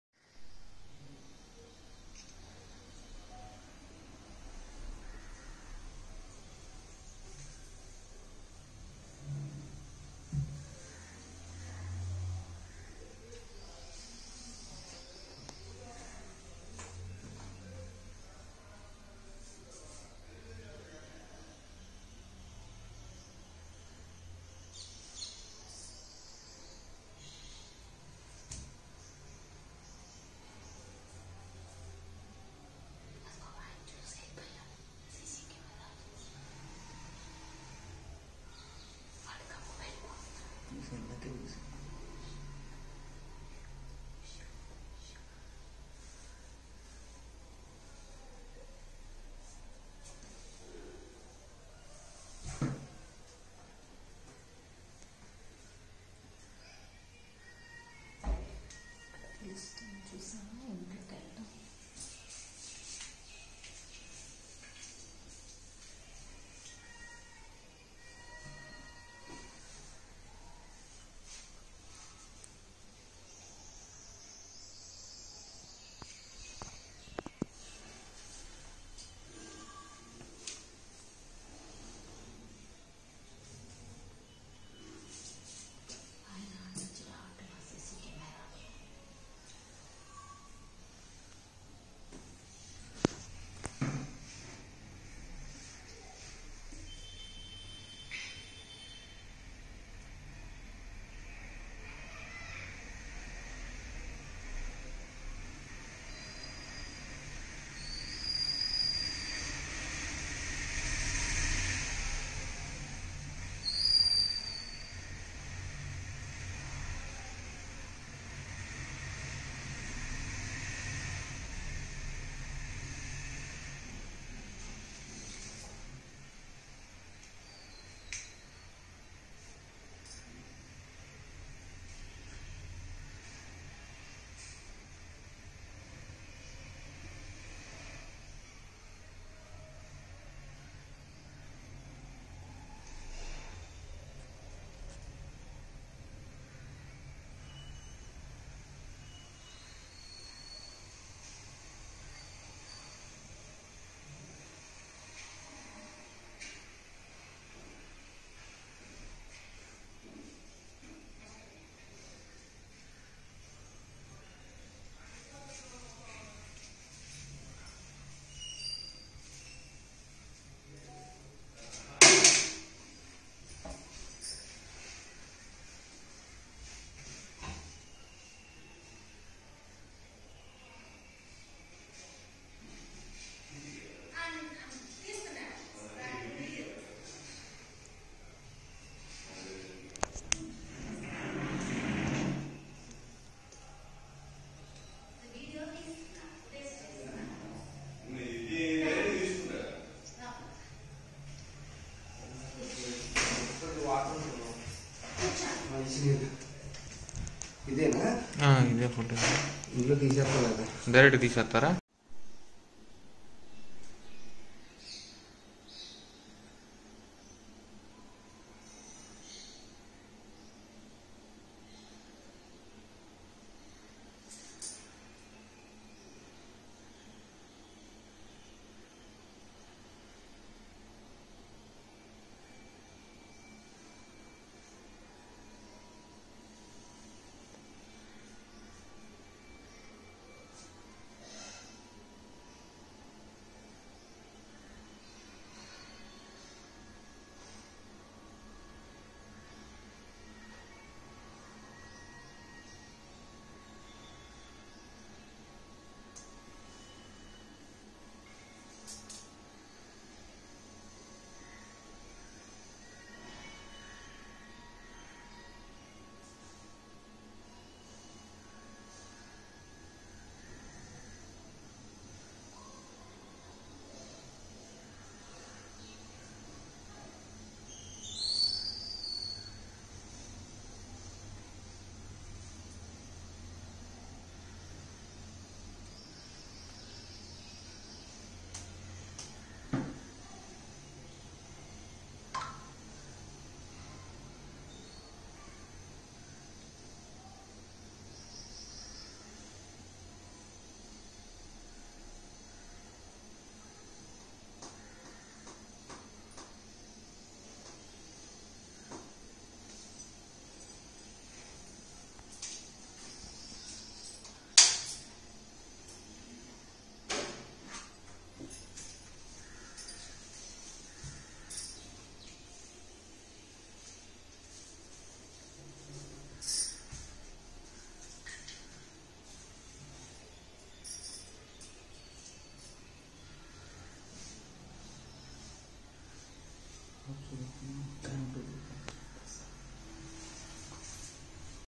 Exclusive Video : AP Police Hulchal in Amaravati Farmers Houses in Mid Night | AP Capital | CM Jagan
Exclusive Video : AP Police Hulchal in Amaravati Farmers Hoses in Mid Night | AP Capital | CM Jagan | TV5 News Telugu